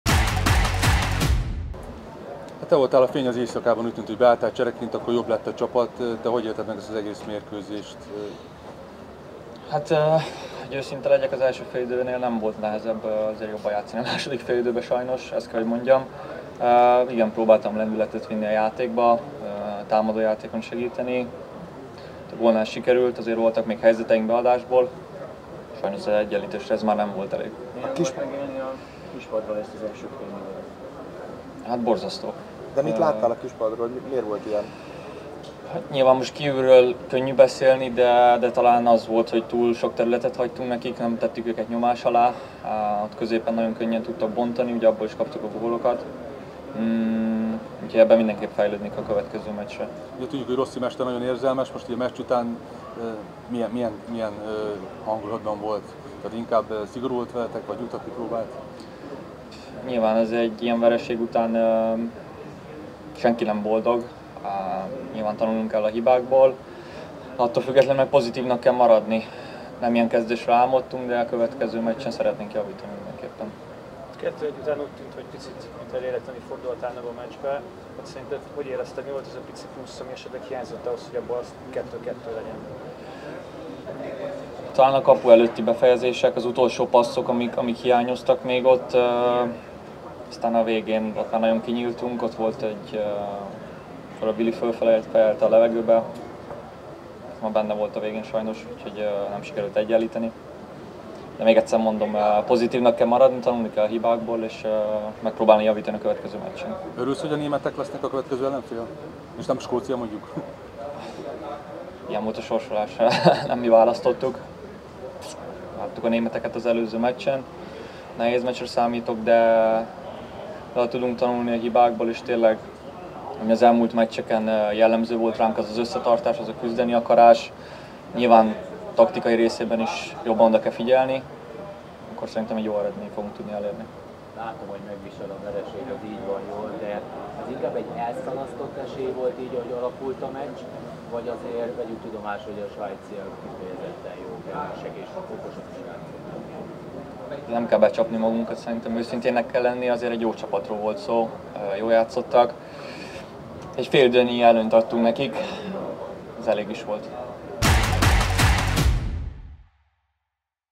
Bolla a Svájc elleni vereség után: az első félidő után nem volt nehéz feljavulni
Bár a szünet után feljavult, így is vereséggel kezdett az Eb-n magyar válogatott. Az első félidőben a tudása alatt játszó együttesnek a hajrában volt esélye az egyenlítésre, ám a hosszabbításban még egy gólt kapott: Svájc 3–1-re nyert. A lefújás után a magyar válogatott szélsője, Bolla Bendegúz értékelt a Sportal TV kamerája előtt a kölni stadion interjúfolyosóján.